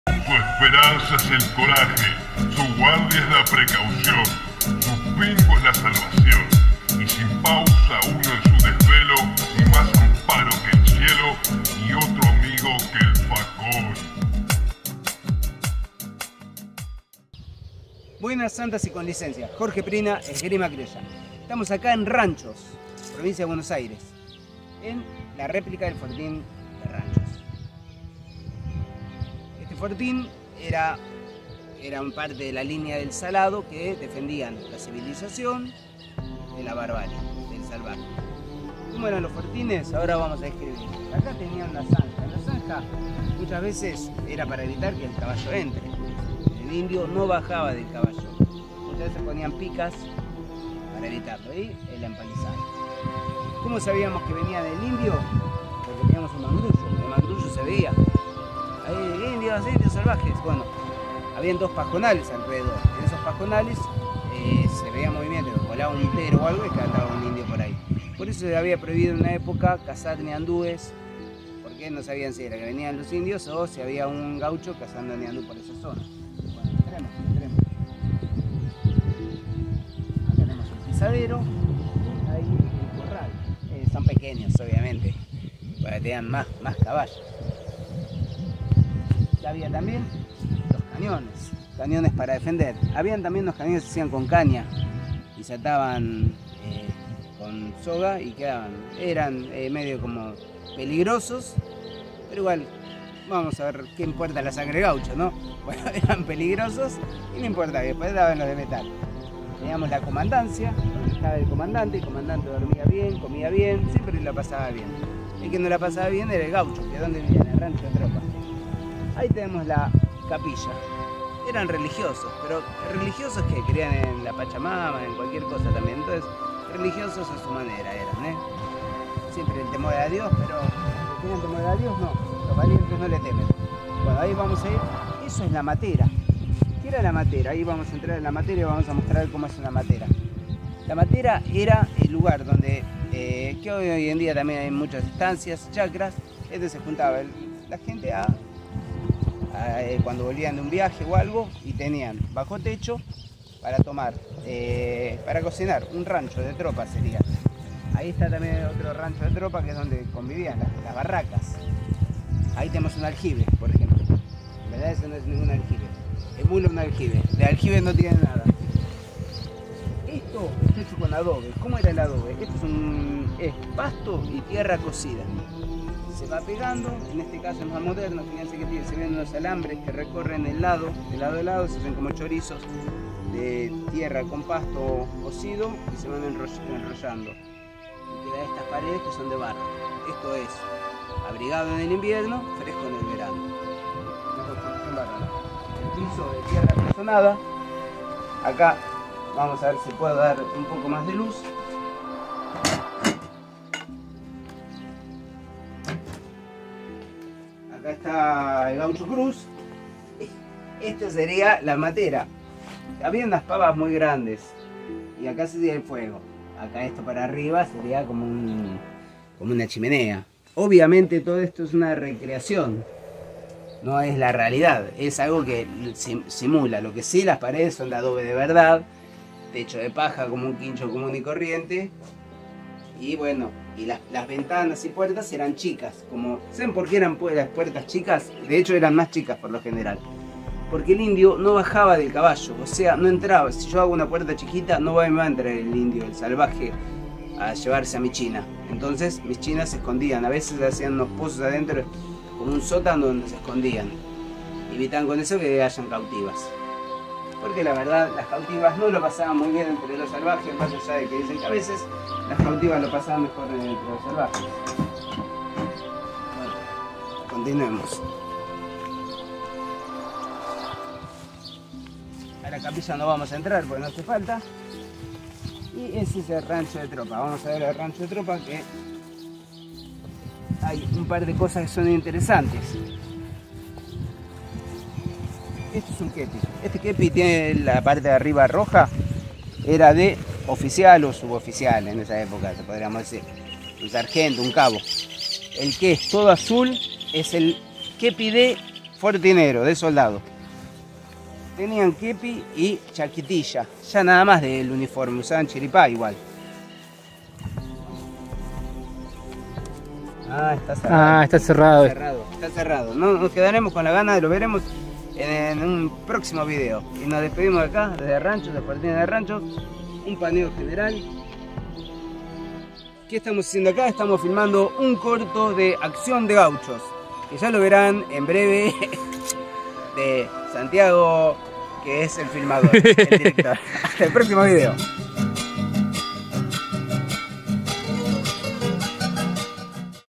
Cómo era un Fortín.
En esta visita Ranchos , provincia de Buenos Aires. Visitamos está recreación de un Fortín donde se ve como era la vida en linea entre civilización y Barbarie